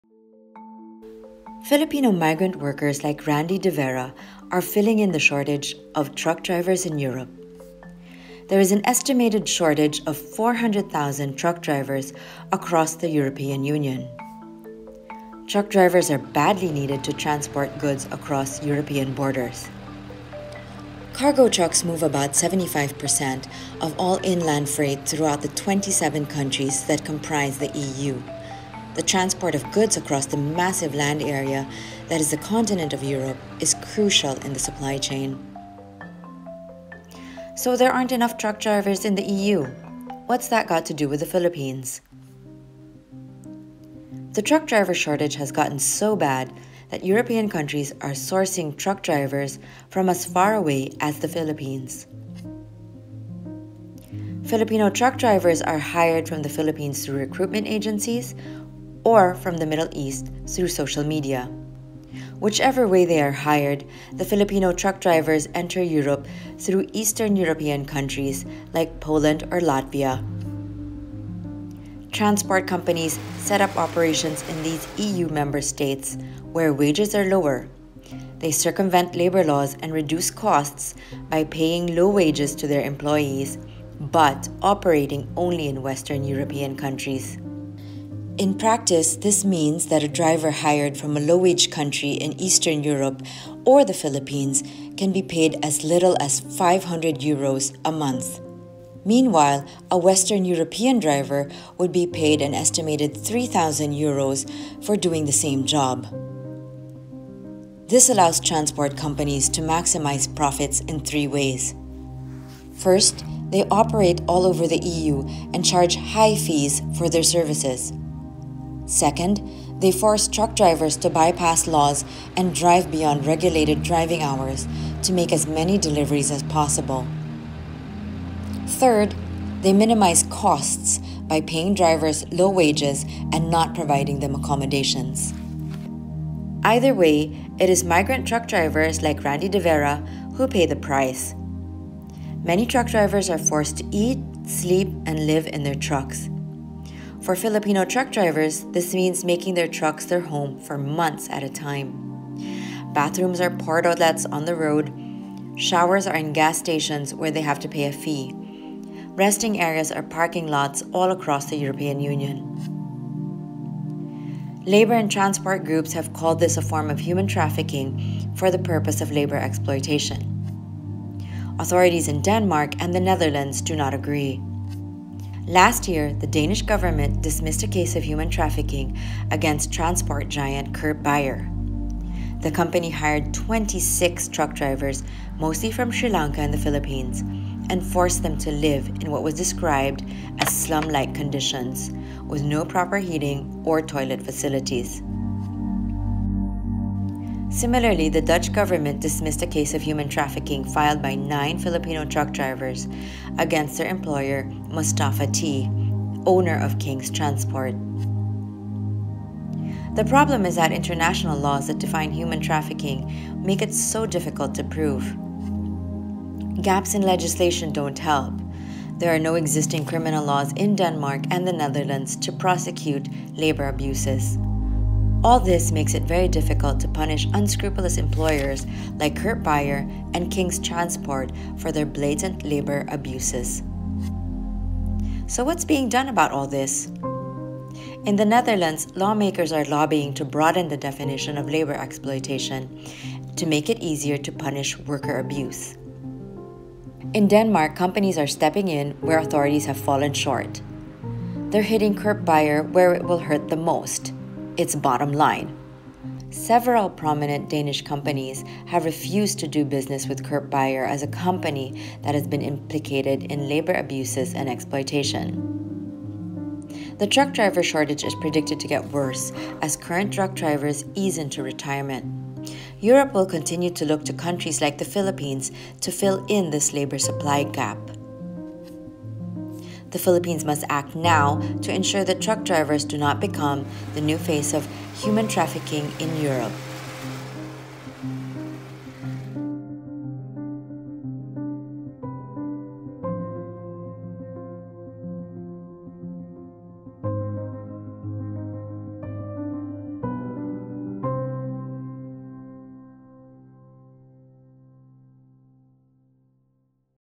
What does the truck driver shortage in Europe have to do with the Philippines?
More videos on Rappler An estimated shortage of 400,000 truck drivers is compounding supply chain issues in Europe. Unscrupulous employers are taking advantage by recruiting truck drivers from as far as the Philippines, and Filipino migrant truck drivers work under exploitative conditions. How has it come to this? Follow Rappler for the latest news in the Philippines and around the world.